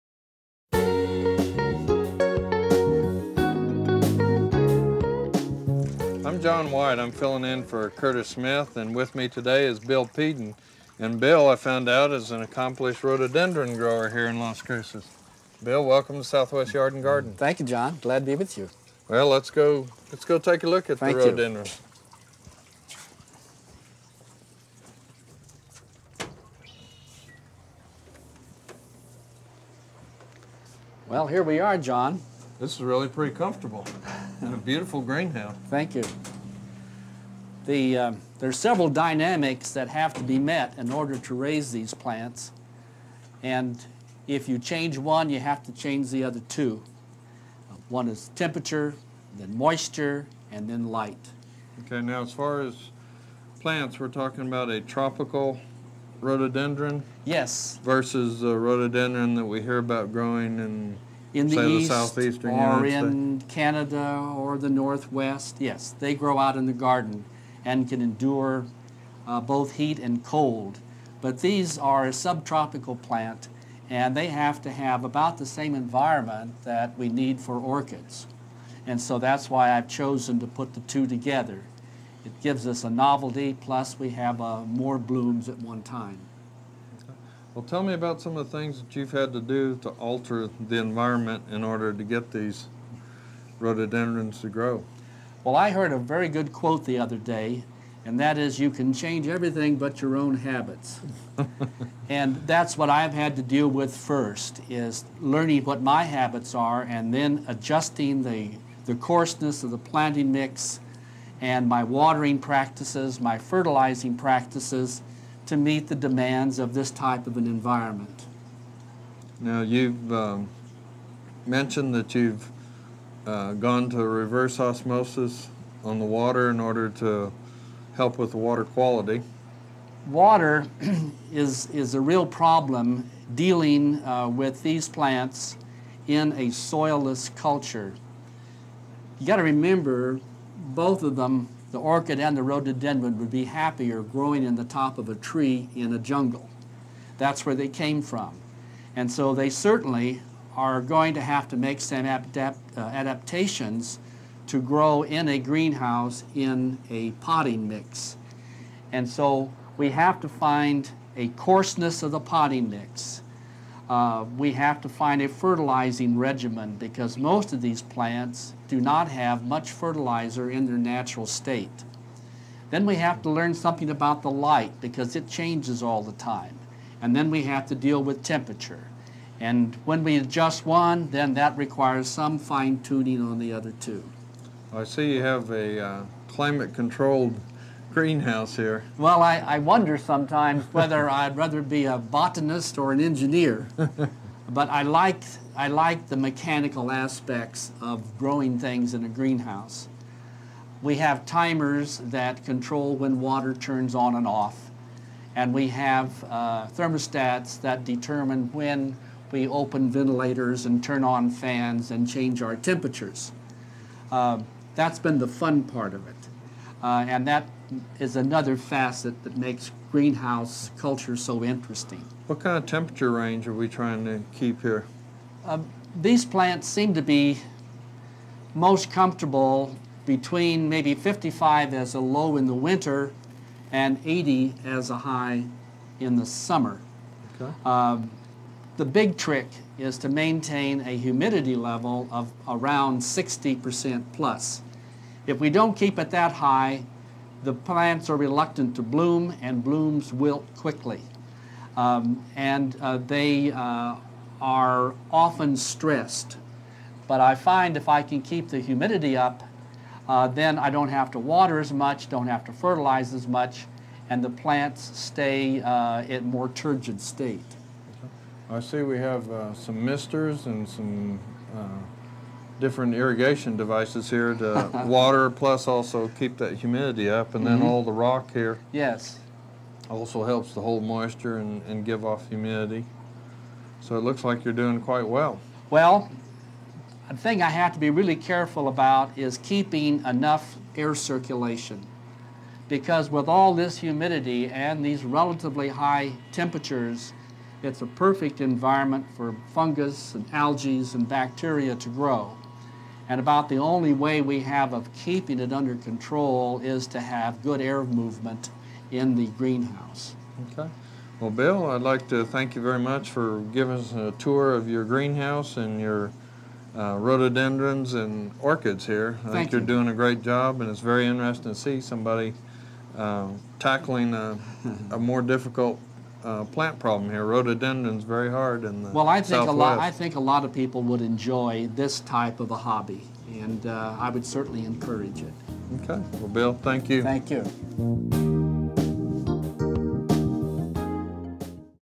How To Grow Rhododendrens In A Greenhouse
Learn how a local gardener grows rhododendrens in his backyard greenhouse.